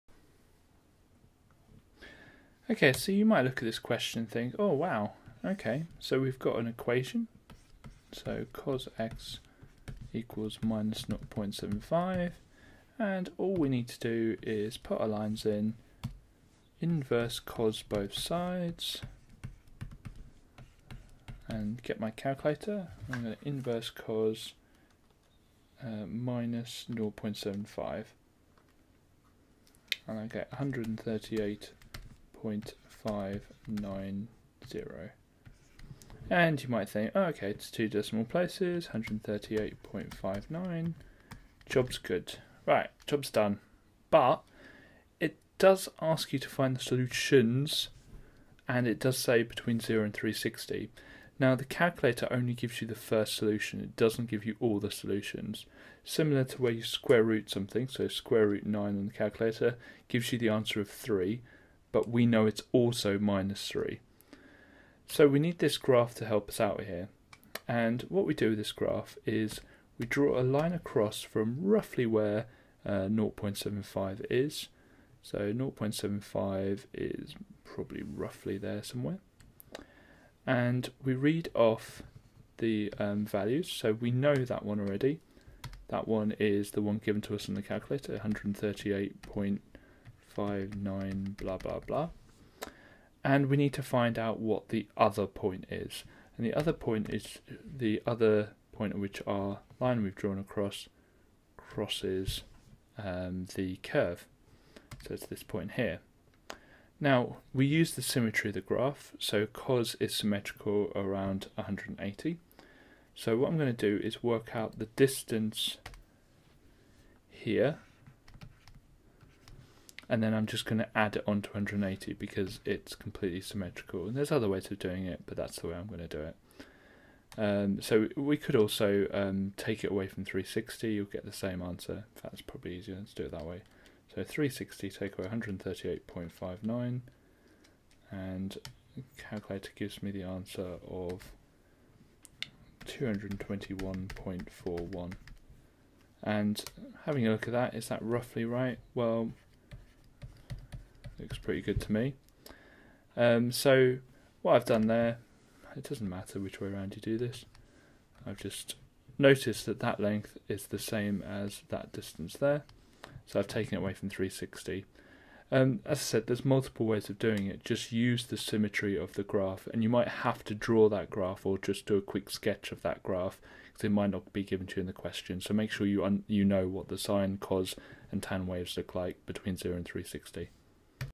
Further Trigonometry: Use Graph to Find Solutions to y = cos x (Grade 9) - GCSE Maths Revision
Topic: Further Trigonometry: Use Graph to Find Solutions to y = cos x